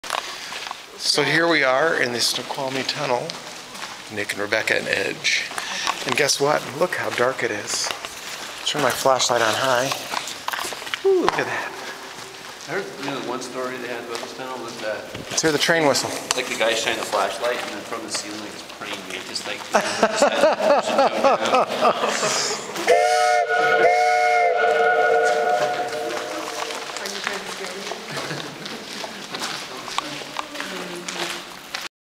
Hyak Tunnel at Snoqualmie Pass
this 2-mile tunnel was built in 1912 for a railroad; today we brought a large 10.6 x 2.7 inch train whistle inside and walked from one end to the other; a cool breeze blew inside the tunnel.